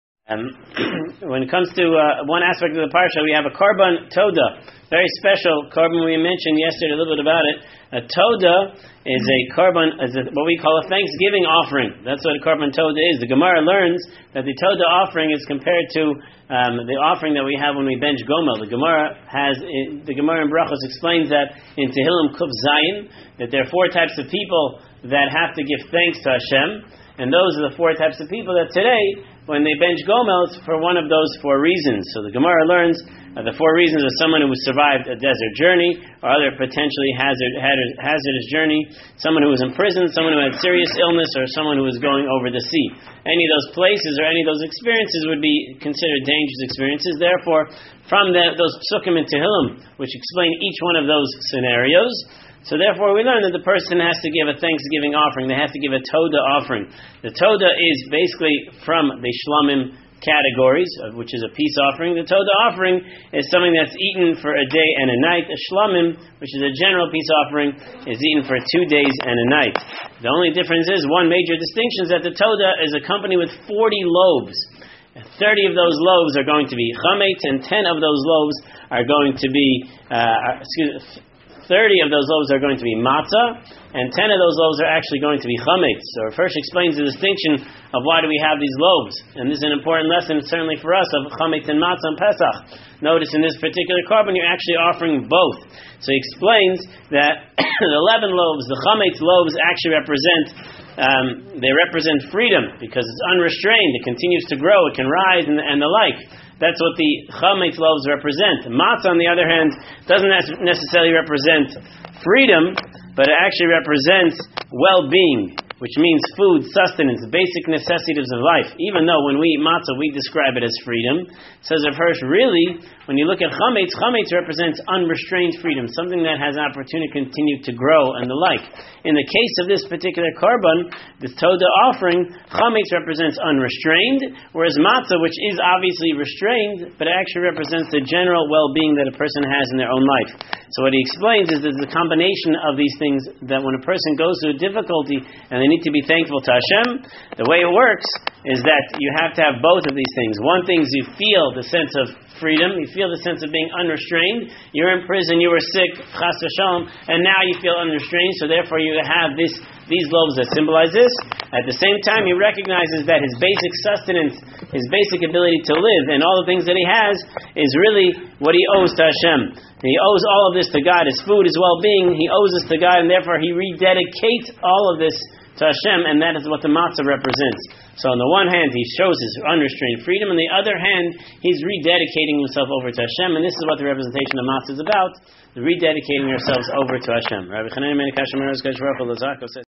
Thanks a Ton! by Rabbi Nachi Klein
Parsha Tzav (Torah4You)
G‑d instructs Moses to command Aaron and his sons regarding their duties and rights as Kohanim ("priests") who offer the korbanot (animal and meal offerings) in the Sanctuary.
The fire on the altar must be kept burning at all times. In it are burned the wholly consumed ascending offering; veins of fat from the peace, sin and guilt offerings; and the "handful" separated from the meal offering.  The Kohanim eat the meat of the sin and guilt offerings, and the remainder of the meal offering. The peace offering is eaten by the one who brought it, except for specified portions given to the kohen. The holy meat of the offerings must be eaten by ritually pure persons, in their designated holy place and within their specified time.